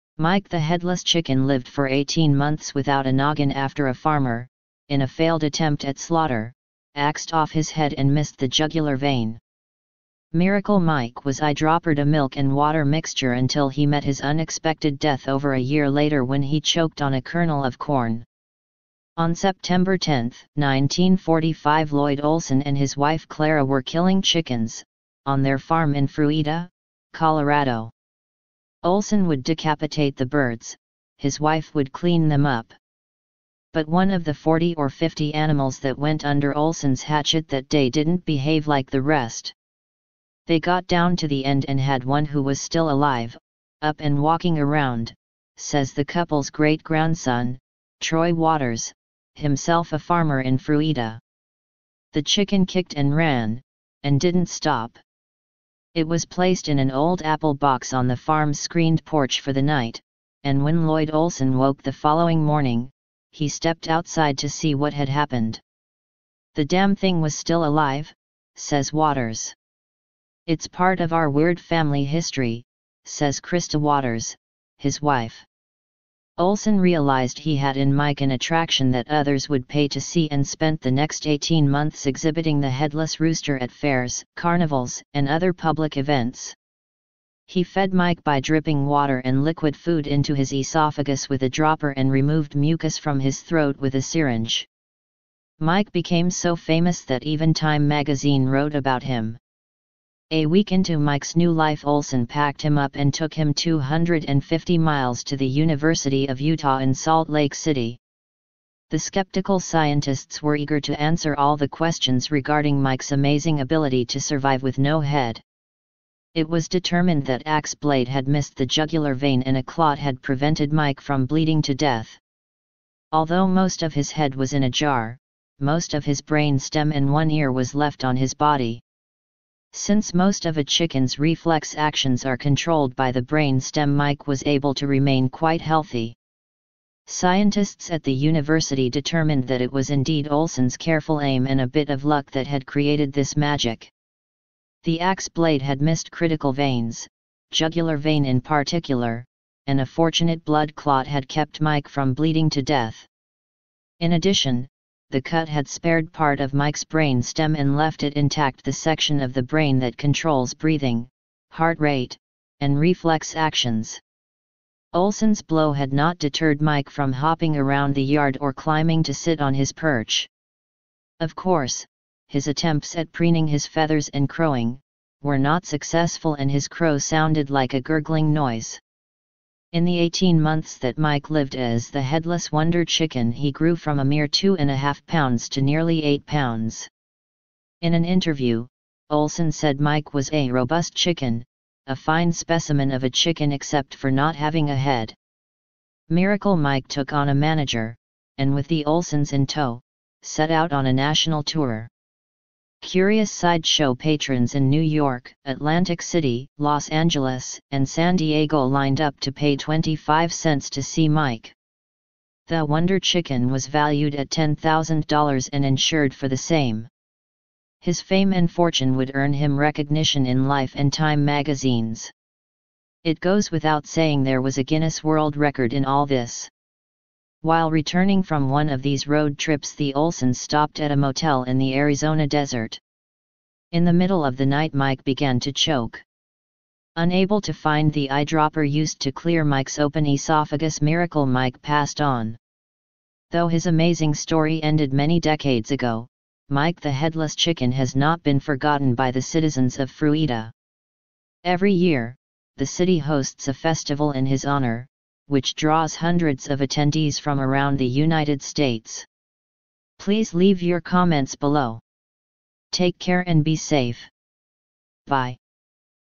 Miracle Mike: The Chicken Lived For 18 Months Without A Head
In this DVJ video, we will be discussing about Headless Chicken lived for 18 months without head.
DVJ gets you closer to the information that matters a life of touch.